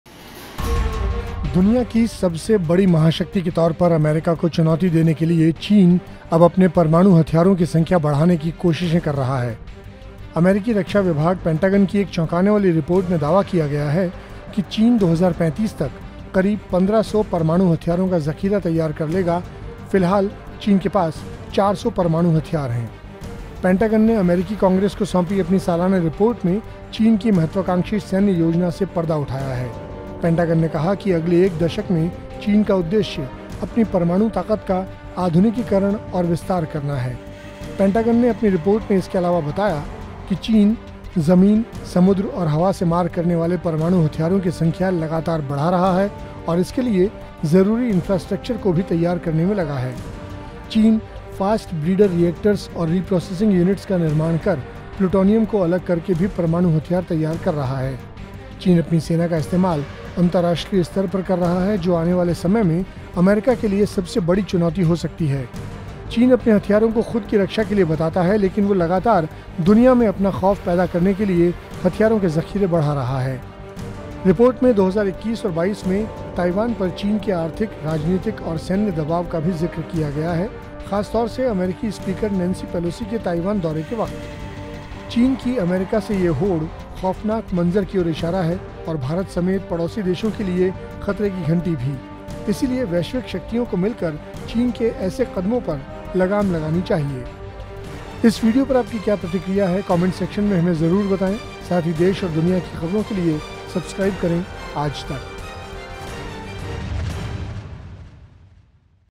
China 2035 तक अपने nuclear weapons की संख्या बढ़ाकर 1,500 कर लेगा -US Department of Defense, Pentagon
China has developed 400 nuclear warheads till now and is expanding its arsenal to 1,500 weapons by the middle of the next decade, a major Pentagon report has revealed. China is trying to challenge the US as the world's top super power.

अमेरिकी रक्षा विभाग- पेंटागन की एक चौंकाने वाली रिपोर्ट में दावा किया है कि चीन 2035 तक करीब 1500 परमाणु हथियारों का जखीरा तैयार कर लेगा. फिलहाल, चीन के पास 400 परमाणु हथियार हैं. दुनिया की सबसे बड़ी महाशक्ति के तौर पर अमेरिका को चुनौती देने के लिए चीन ऐसी कोशिशें कर रहा है.